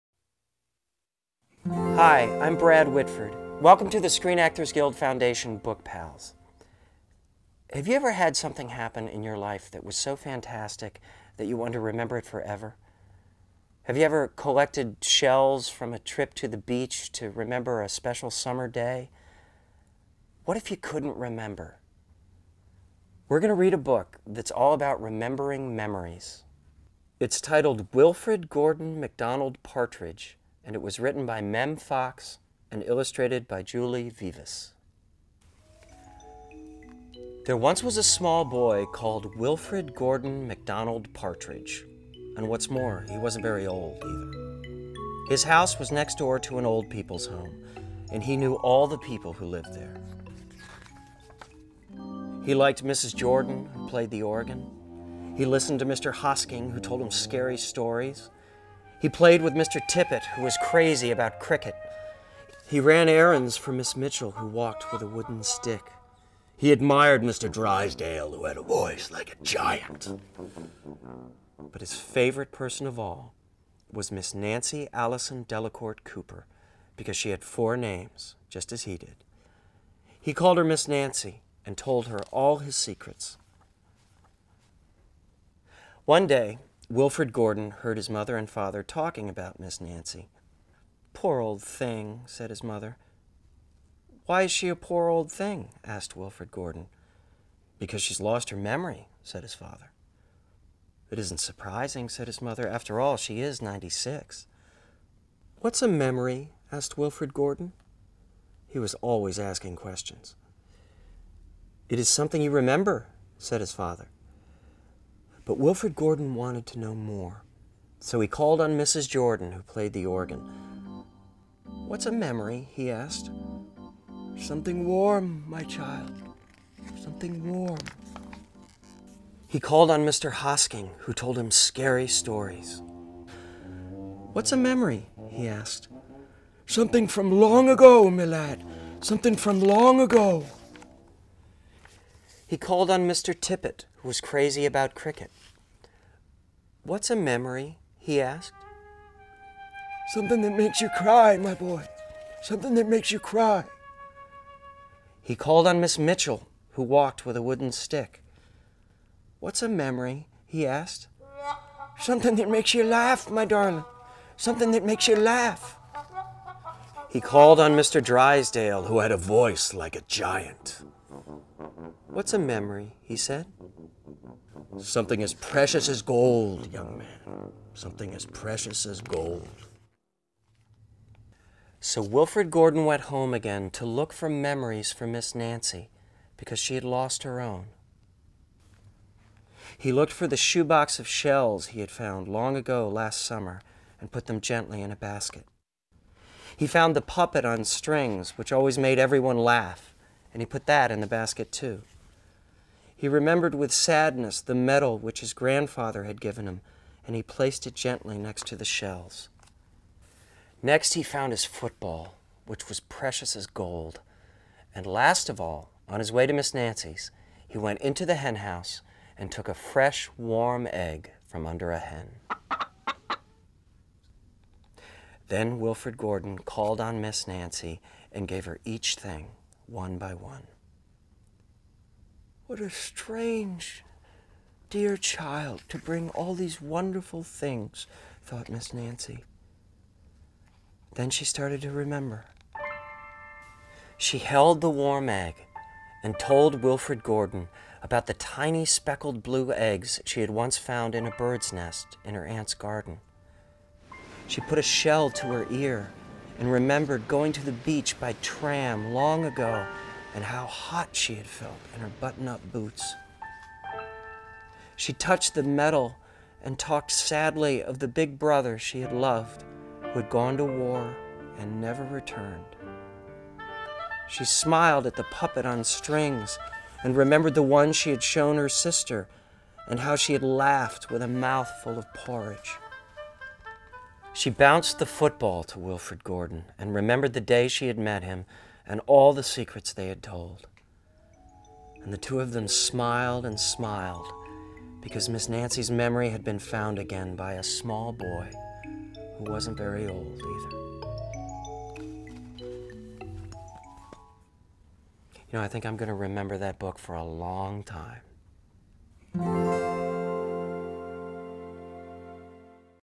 'Wilfrid Gordon McDonald Partridge' read by Bradley Whitford
'Wilfrid Gordon McDonald Partridge' is written by Mem Fox, illustrated by Julie Vivas and read by Bradley Whitford.

Wilfrid lives next to a retirement home, and his favorite old person is 96-year-old Miss Nancy. Everyone says Miss Nancy has lost her memory, and despite the fact that Wilfrid doesn't even know what a memory is, by accident he helps her find it.